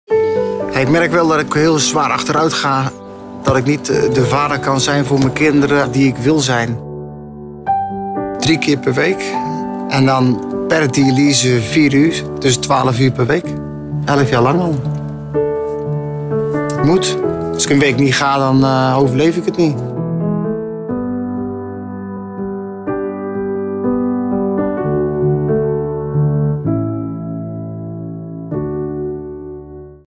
Patrick, nierpatient sinds zijn 17e
Patrick houdt vol. Net als wij. Totdat Patrick en alle andere nierpatiënten weer de vrijheid hebben om gewoon te kunnen leven. Help mee. Doneer op nierstichting.nl

TV commercial 30 sec